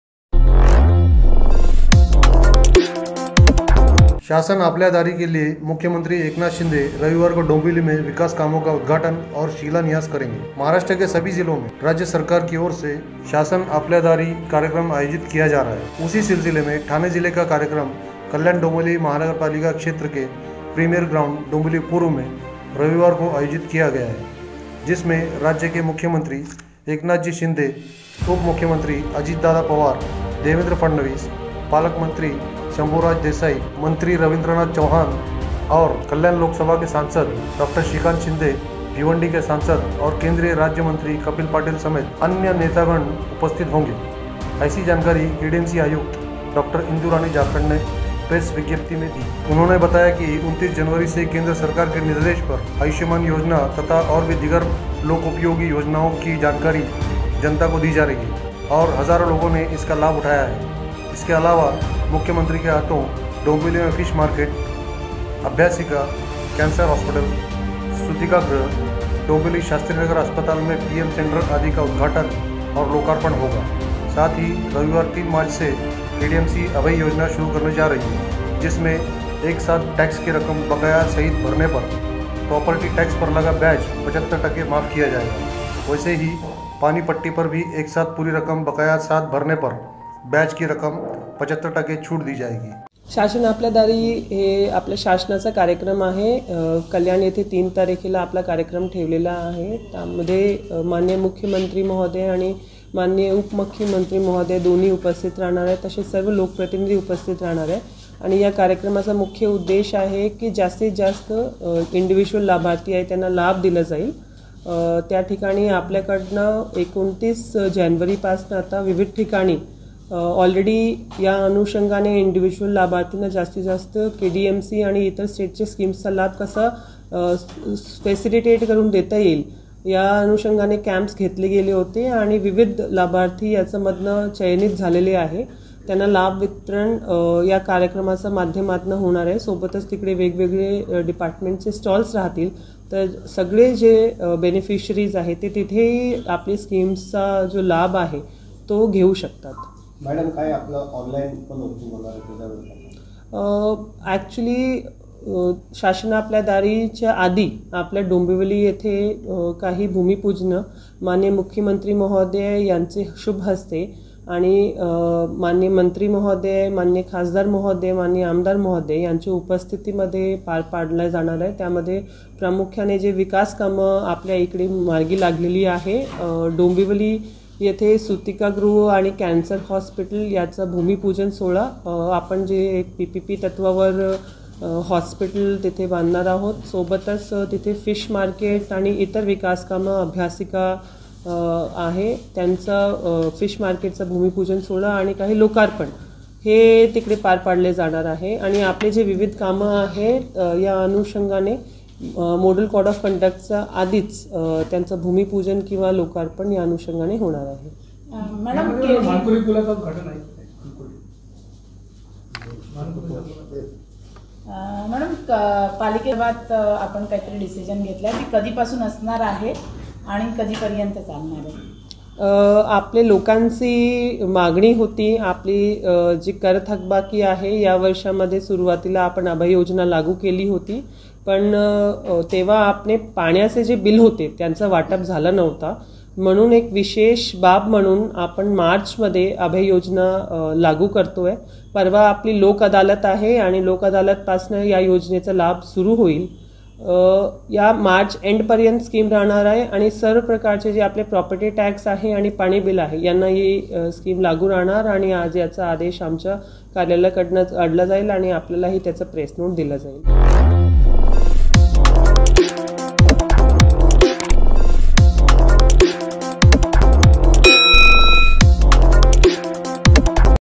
KALYAN | शासन आपल्या दारी,CM एकनाथ शिंदे कल डोंबिवली में विकास कामों का उद्घाटन और शिलान्यास करेंगे।
*अपने परिसर की खबरे दिखाने के लिए इस नंबर पर WhatsApp या Call करके सम्पर्क करे -
*AAJ KA SANDESH NEWS*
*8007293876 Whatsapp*